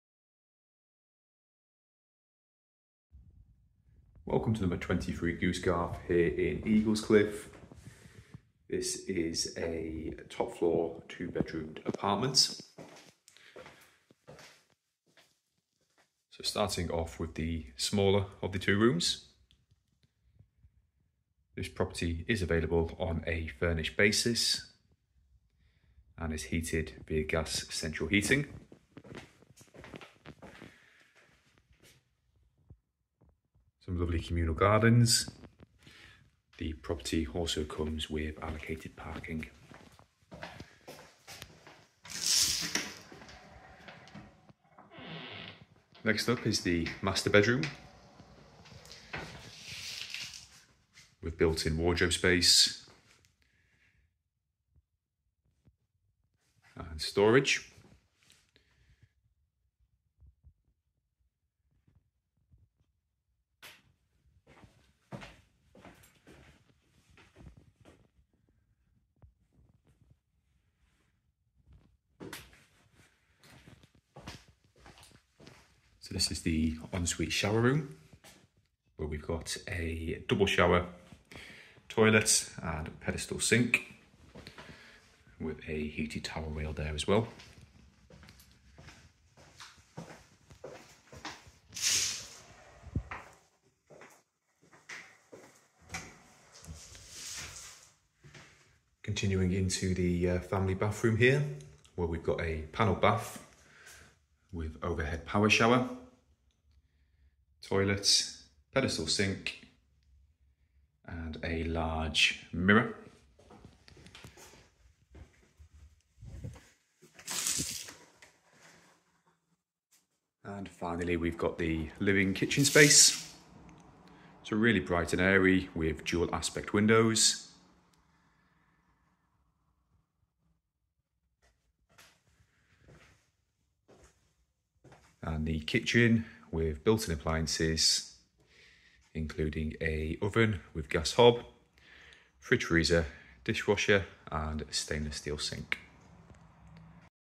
23 Goose Garth, Eaglescliffe
Property available to rent on Goose Garth, Eaglescliffe.